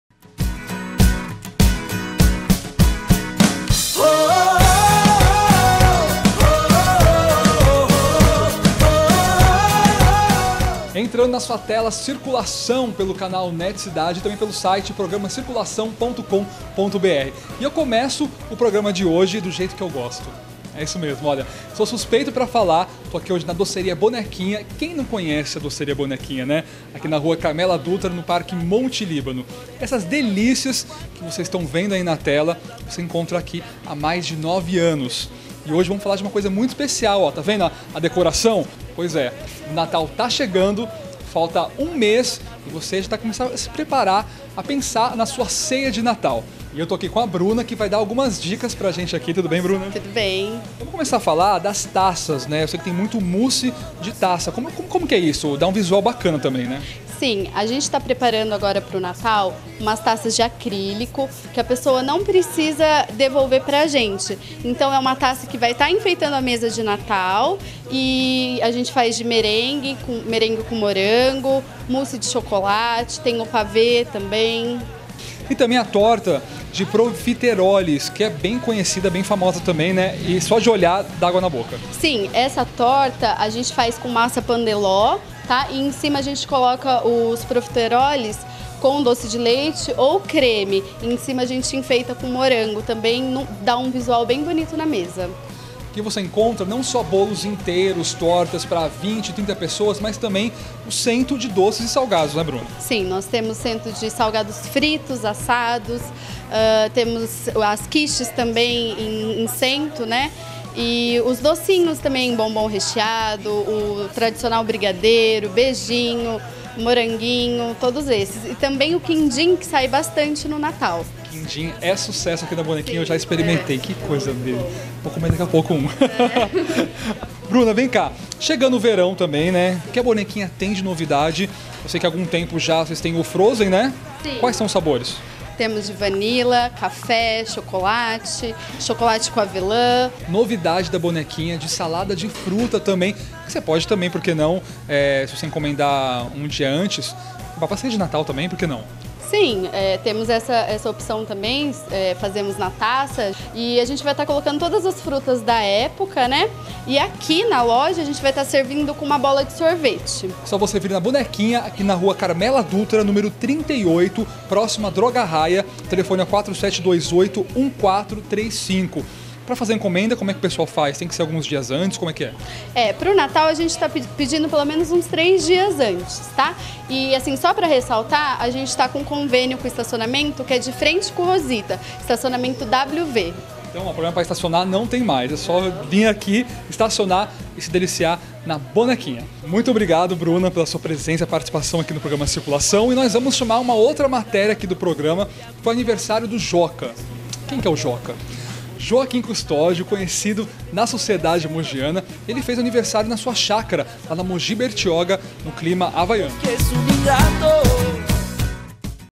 Doces para a sua ceia de Natal
Bruna Goshn dá dicas de pratos deliciosos para as comemorações de fim de ano.